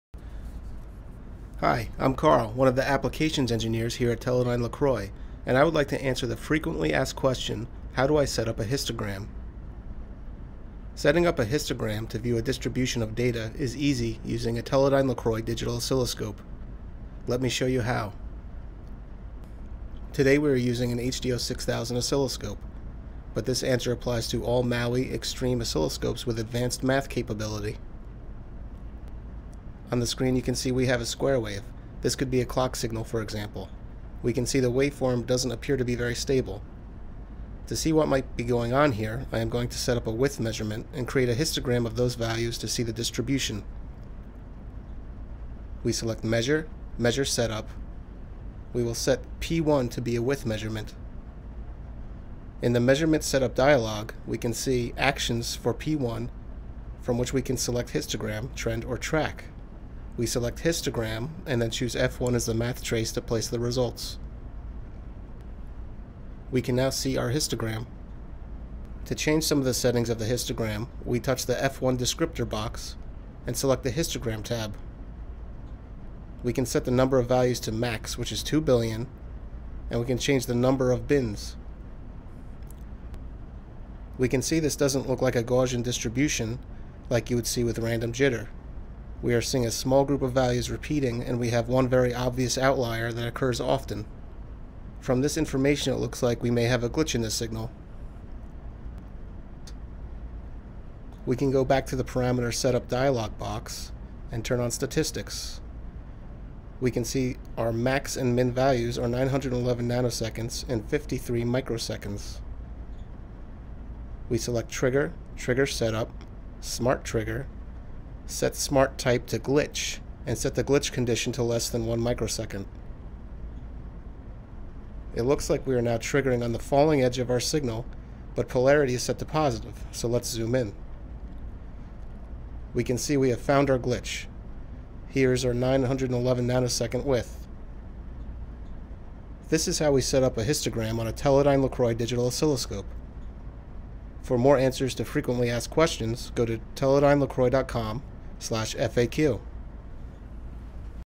Oscilloscope FAQ - Histogram | Teledyne LeCroy
In this video FAQ we answer the question "How Do I set up a Histogram on a Teledyne LeCroy Maui/Xstream Oscilloscope?"